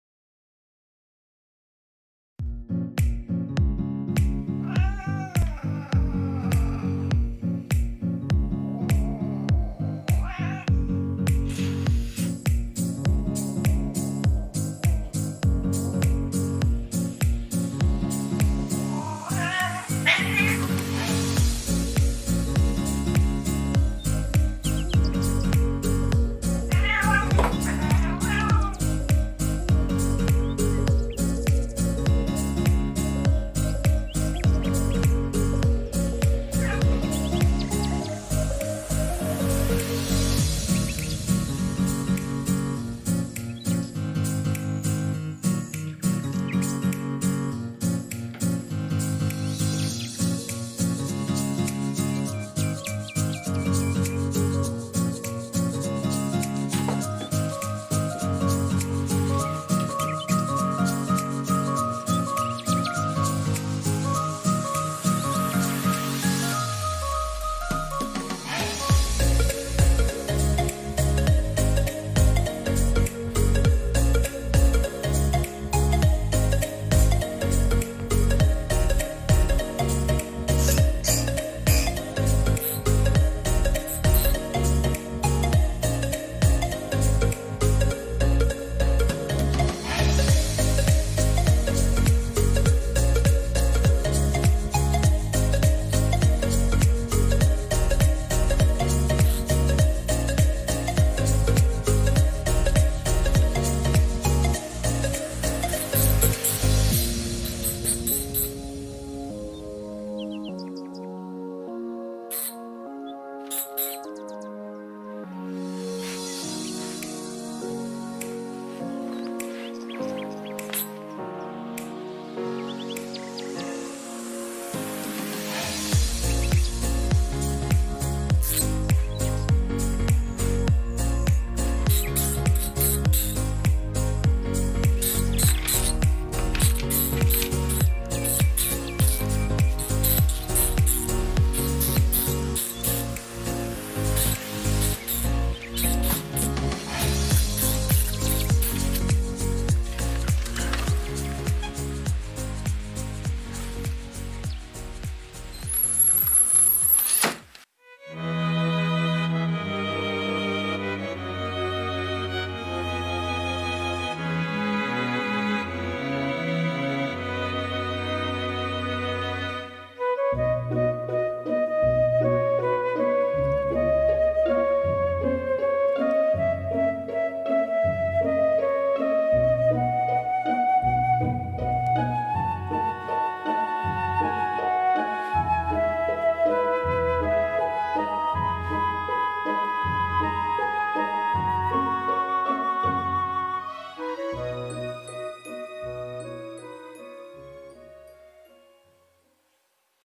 【庭猫】つづら夜の居間で大騒ぎ
猫同士でバトルをしたり、モグラをおもちゃにしたりで、夜の室内は大騒ぎ。